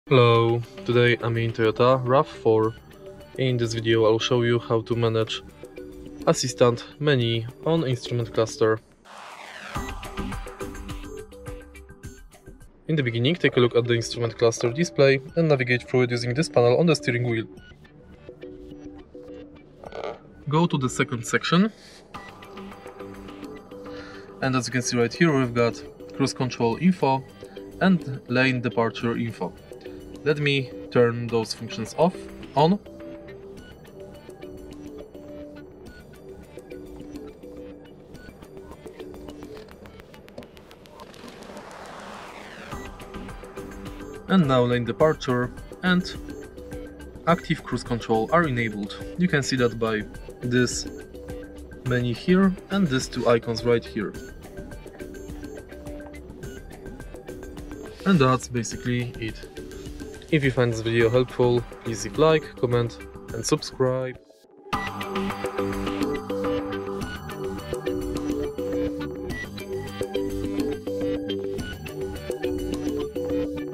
How to Enter Assistant Display on Speedometer in Toyota RAV4 V ( 2018 – now )
Find out more info about Toyota RAV4 V ( 2018 – now ) :
Would You like to know how to Enter Assistant Display on the Speedometer in Toyota RAV4 V? Thanks to this function in Your car You'll be able to manage all car assistants in no time. Follow the video we've provided above to learn how to Open Assistant Display on the Speedometer in Toyota RAV4 V!

How to Open Assistant Display on Speedometer? How to Enter Assistant Display On Speedometer?

Years of Production:

2018 – now

Generation:

 V Generation - ( 2018 – now )

Series:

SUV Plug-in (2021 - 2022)
SUV Plug-In Facelifting (2022 - now)
SUV Facelifting (2022 - now)